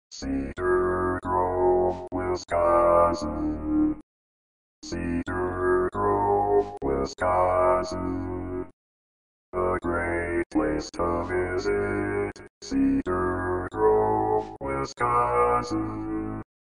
How to Say or Pronounce USA Cities — Cedar Grove, Wisconsin
This video shows you how to say or pronounce Cedar Grove, Wisconsin.

A computer said Cedar Grove, Wisconsin. How would you say Cedar Grove, Wisconsin?